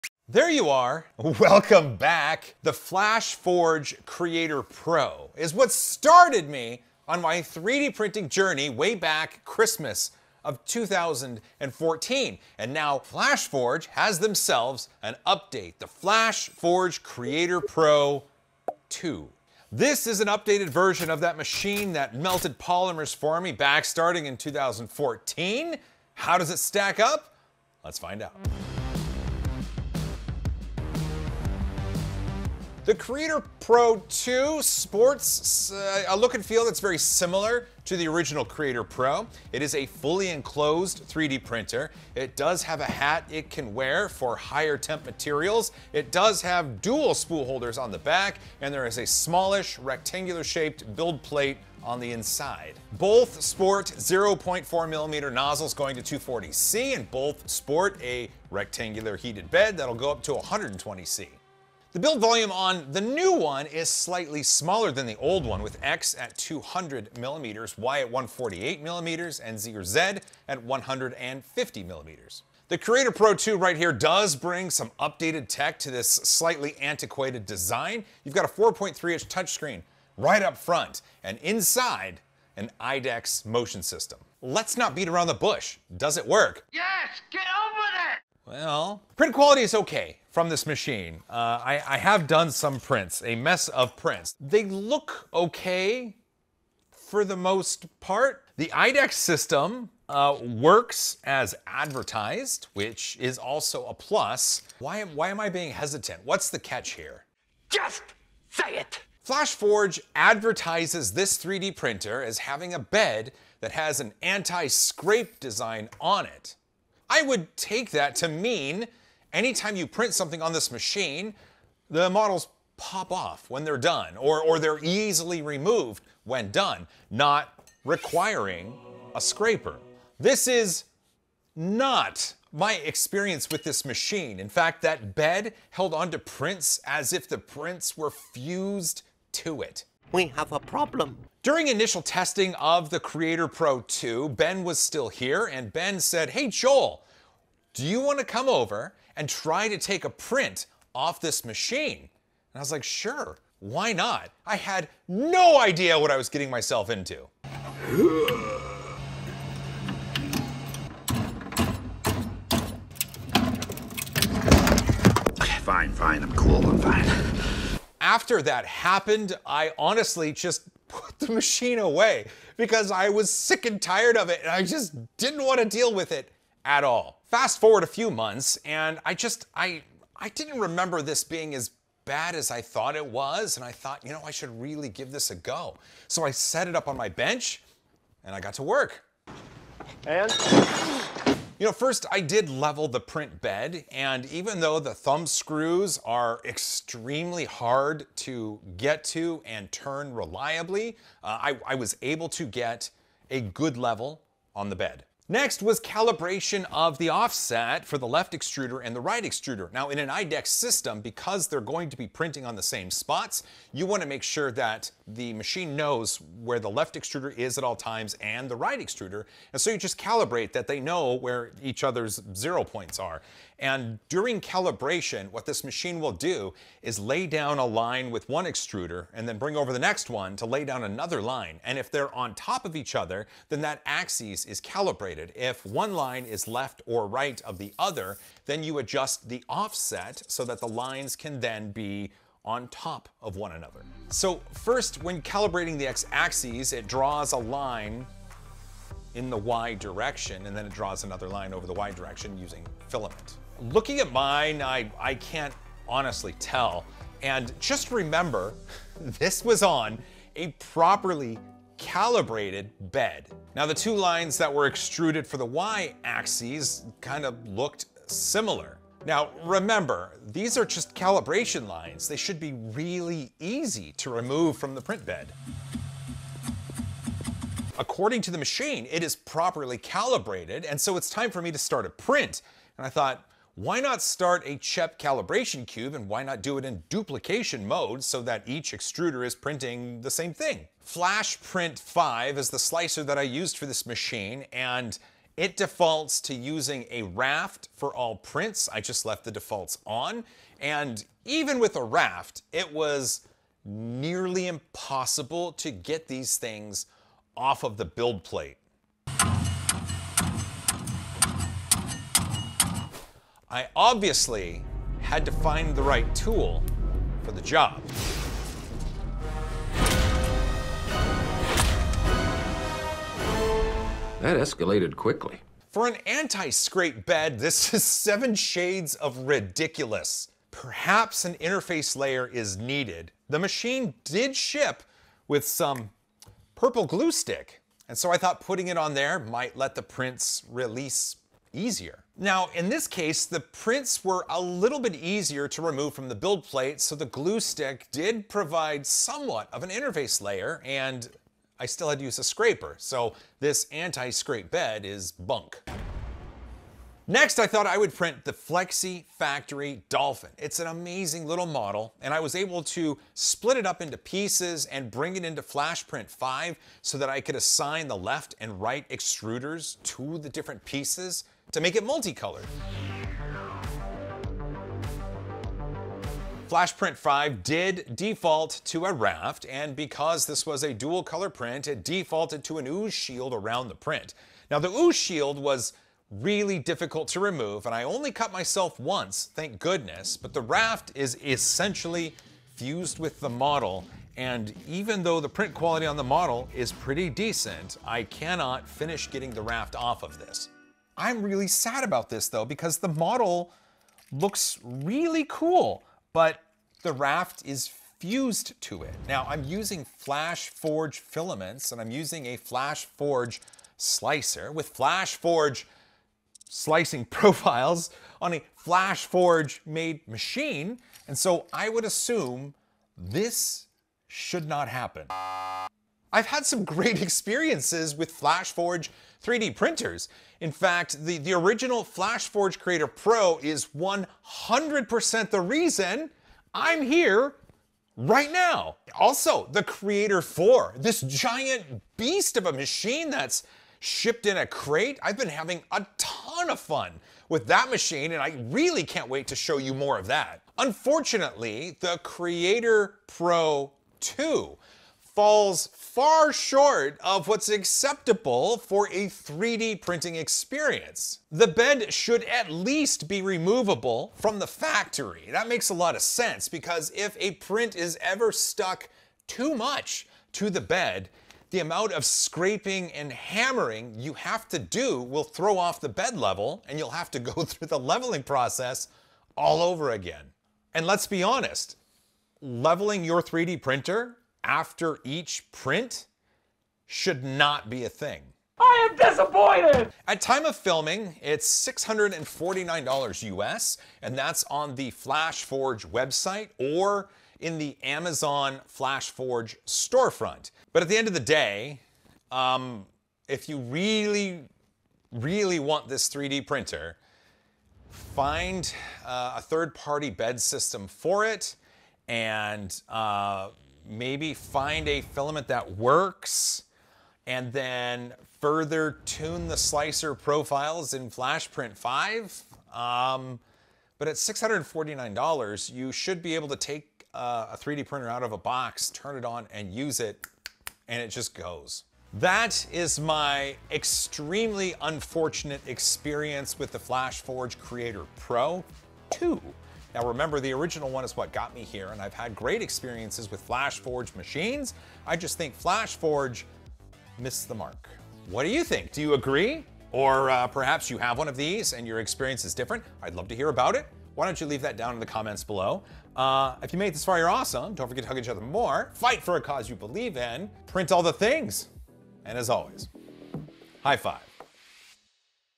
Did Flashforge Miss the Mark?
The Flashforge Creator Pro 2 promises an upgrade packed in an antiquated design. However, does it perform? In my review, I'll answer that question.

SUPPORT! 😍

GEAR! 🎥🎦📷

THE TEAM! 🤟

Find Me Socially!

Want to send me something?
3D Printing Nerd
USA